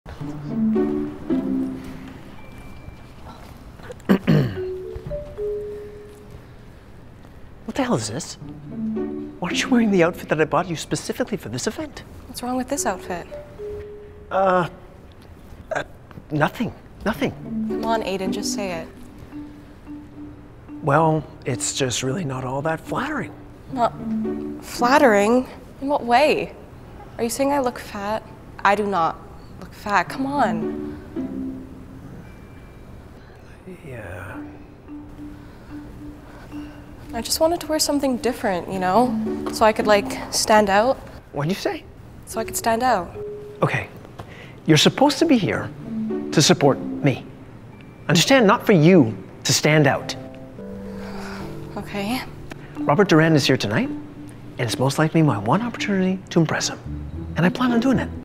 Toxic Boyfriend Humiliates Girlfriend In Public
Will this toxic boyfriend learn his lesson?